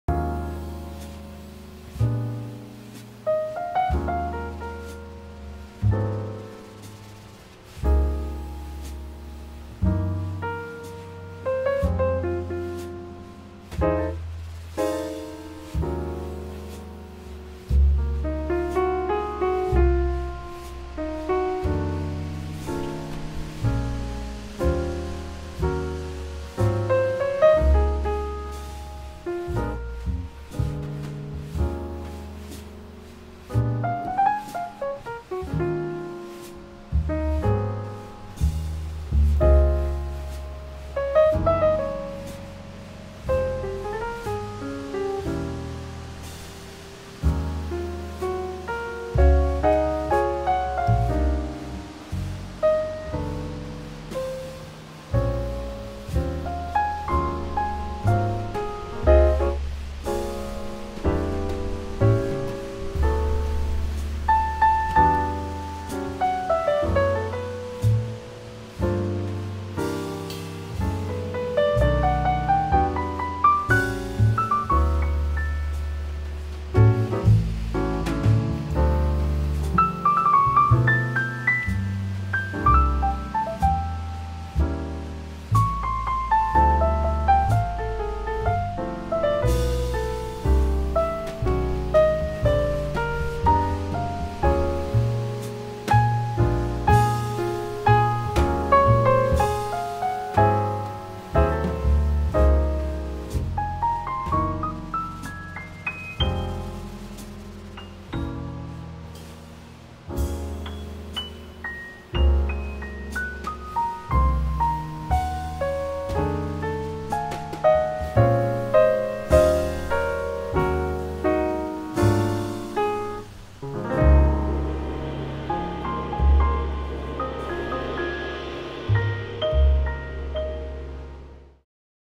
Canon C100 Mark 2 Clog Test Footage Ungraded vs Graded
Did some CLOG test shooting on a rainy Sunday. I was recording straight to the SD cards so the overall resolution of the images is weaker than had I been shooting with a Ninja 2 external recorder. Did some quick grading in AVID Media Composer.